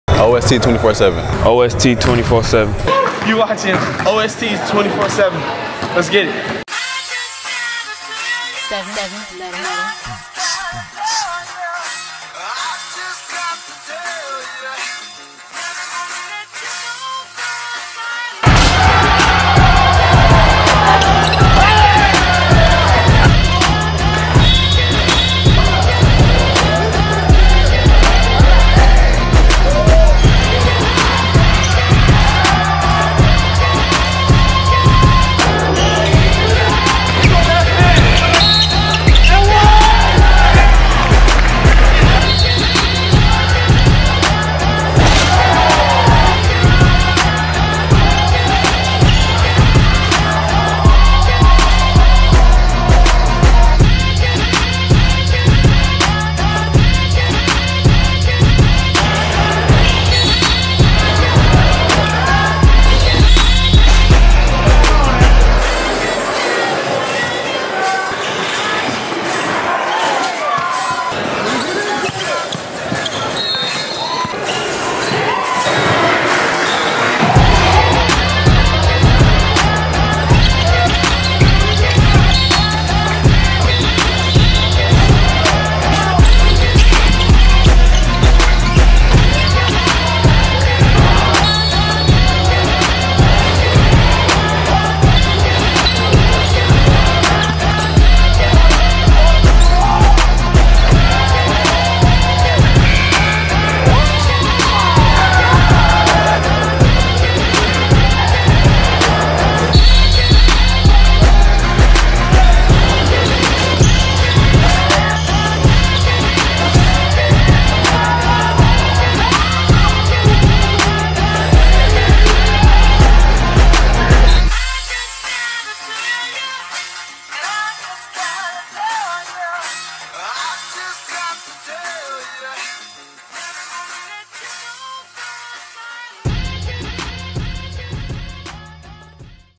Emmitt Williams aka Young Vince Carter | E1T1 Mixtape
Emmitt Williams is known to leave you standing with your mouth open like "Wowwww" with some of his crazy in game dunks. Check out Emmitt Williams E1T1 mix and tell us what you think.

DON'T FORGET TO CLICK THAT BELL ABOVE TO GET NOTIFICATIONS FOR FUTURE VIDEOS

Thanks for rocking with US!!!